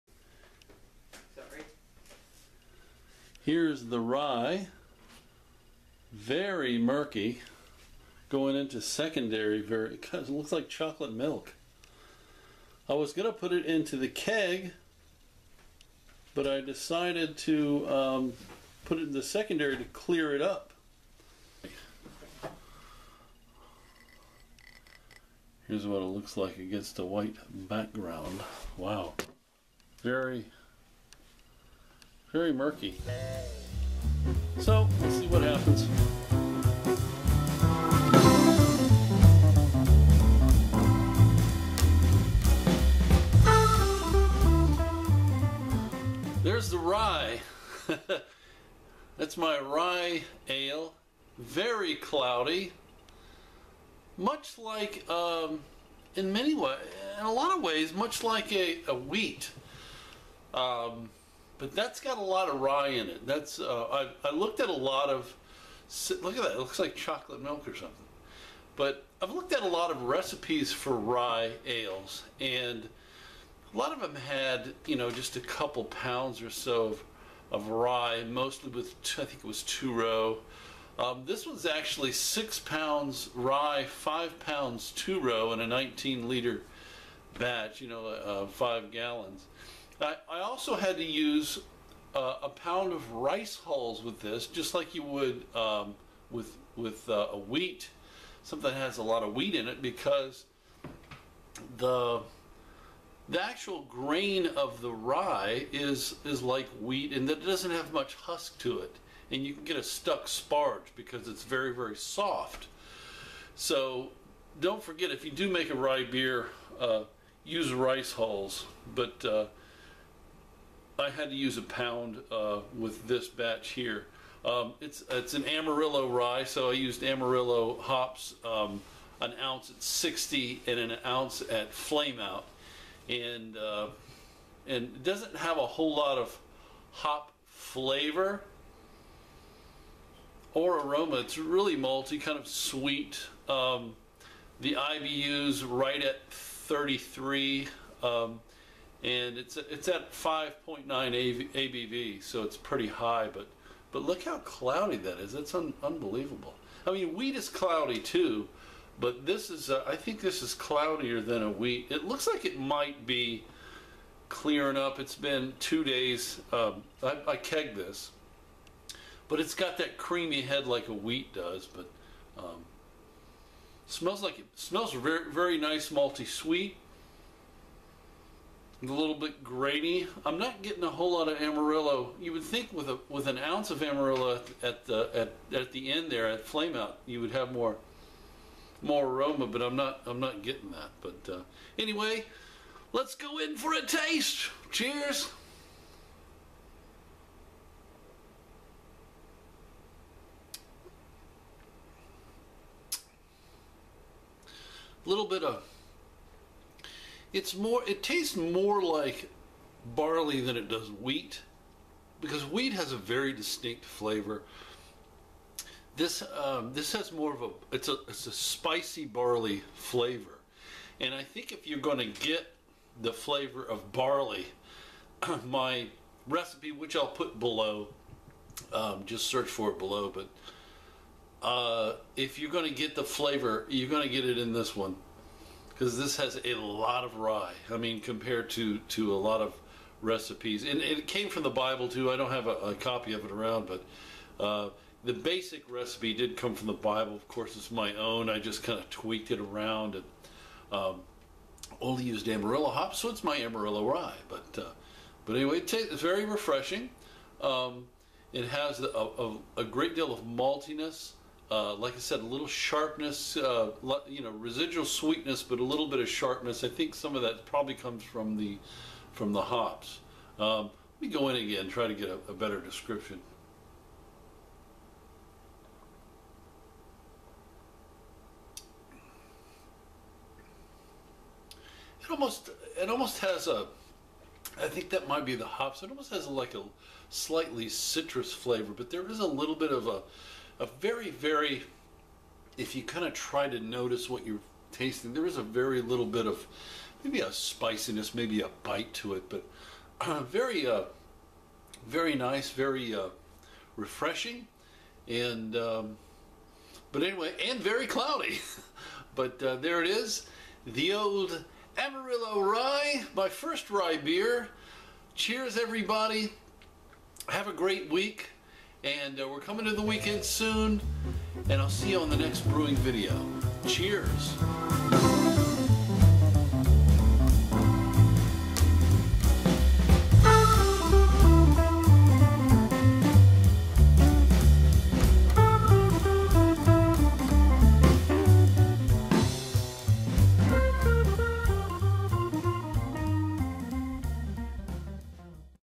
Amarillo Rye 5.9% ABV
This is my first rye ale! Came out really thick and cloudy--not the prettiest beer, but tastes nice with a balance of malty sweetness and a little hop bitterness. My idea for this beer originally came from "Home Brew Beer"  A.K.A."The Bible" on page, 189 "Rye Beer."

HOME BREW RECIPE:
Title: Amarillo Rye
Author: homebrew62

Brew Method: All Grain
Style Name: Blonde Ale
Boil Time: 60 min
Batch Size: 5.5 gallons (fermentor volume)
Boil Size: 7.5 gallons
Boil Gravity: 1.041
Efficiency: 70% (brew house)

STATS:
Original Gravity: 1.055
Final Gravity: 1.011
ABV (standard): 5.89%
IBU (tinseth): 33.08
SRM (morey): 4.91

FERMENTABLES:
6.1 lb - American - Rye (48.4%)
5.5 lb - American - Pale 2-Row (43.7%)
1 lb - Rice Hulls (7.9%)

HOPS:
1 oz - Amarillo, Type: Pellet, AA: 8.8, Use: Boil for 60 min, IBU: 33.08
1 oz - Amarillo, Type: Pellet, AA: 8.8, Use: Whirlpool for 0 min at  °F

YEAST:
Fermentis / Safale - American Ale Yeast US-05
Starter: No
Form: Dry
Attenuation (avg): 81%
Flocculation: Medium
Optimum Temp: 54 - 77 F
Fermentation Temp: 70 F